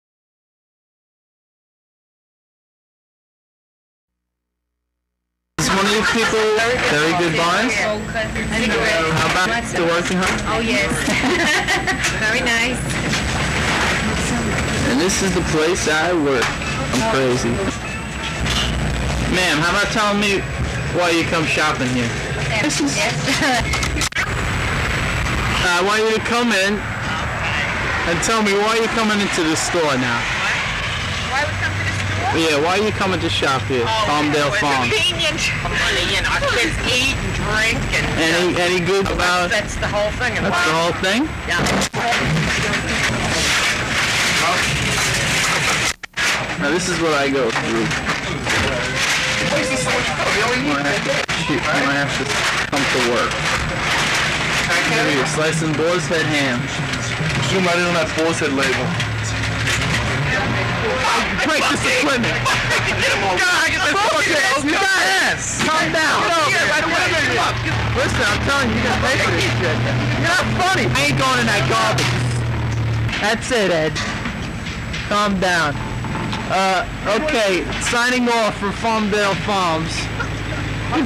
A look back at Farmdale Farms Farmingdale NY 1978 - Mark Chiomastro RIP
I salvaged what I could of this raw black and white footage shot for a beginning TV class at NYU in 1978. This was the grocery store / deli I worked at with Timmy Considine , John Romano , and Rose the cashier. Our boss and owner was the late, great, Mark Chiomastro. We had a lot of fun back in the day. Mark was a really great guy and always encouraged us to move on and not be stuck working at a deli all our lives. I still think about him every time I drive by the place..which is now a flower shop : (